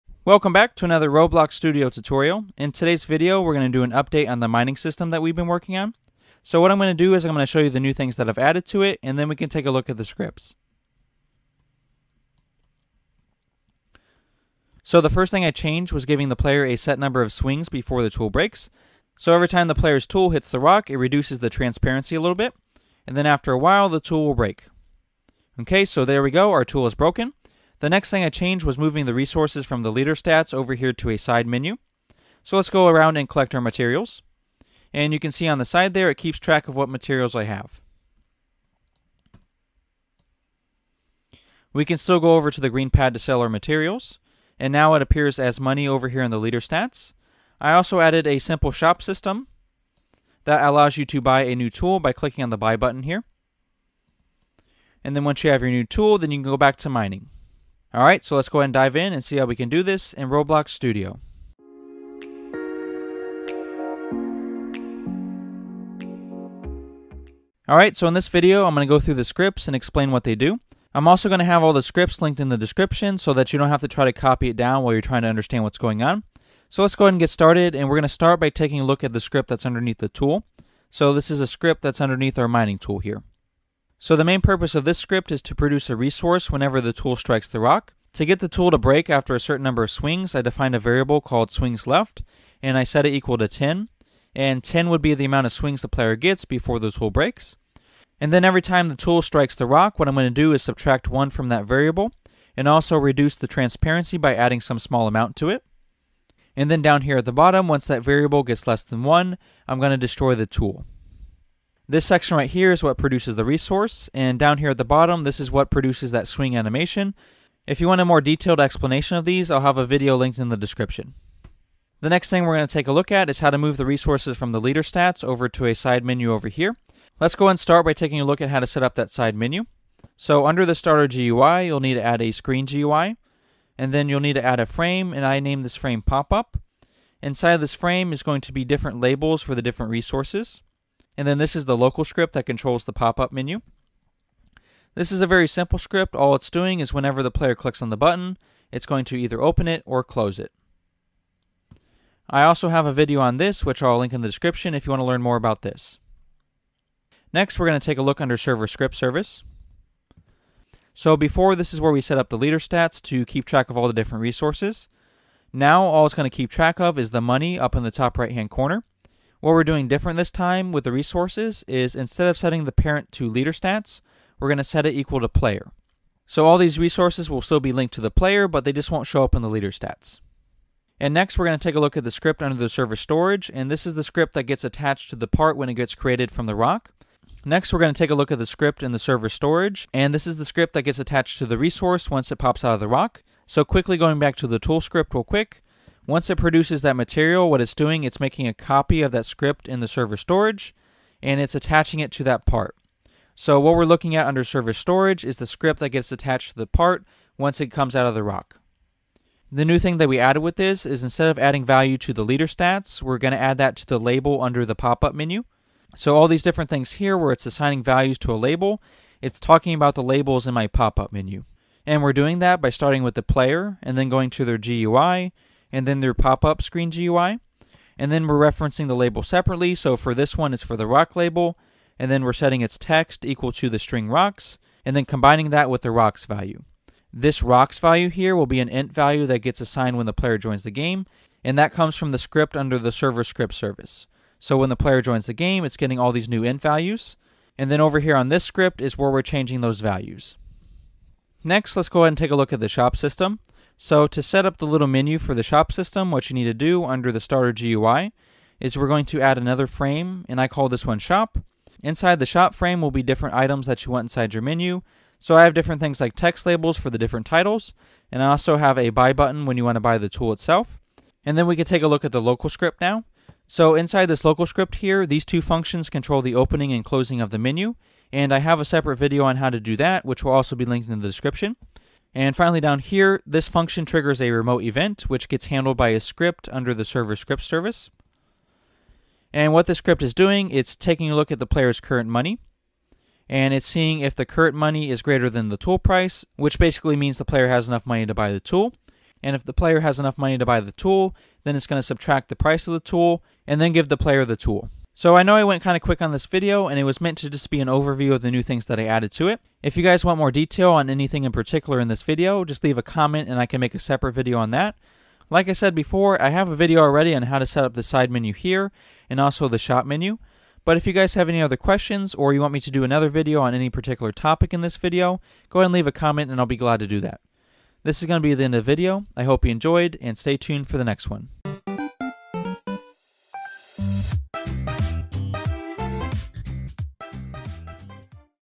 Roblox Studio Tutorial: Mining Game (Update)
In this video I show you how to add a mining system in your Roblox Studio game! This is an update video that show so changes made to the mining system/game.

Other Videos in Series

Pop-up Menu

Shop Menu